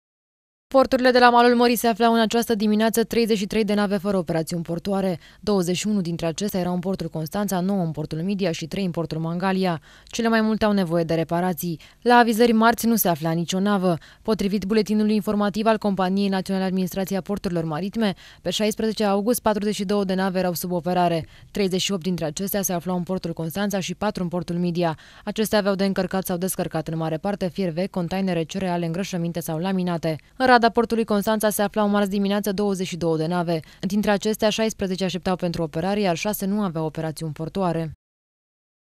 Jurnal portuar
42 de nave sub operare se aflau în această dimineață în porturile de la malul mării. De asemenea, la avizări nu era nicio navă, iar în rada Portului Constanța se aflau 22.  Mai mult, 33 de nave erau fără operațiuni portuare, potrivit Companiei Naţionale Administraţia Porturilor Maritime. Mai multe amănunte, în jurnalul portuar.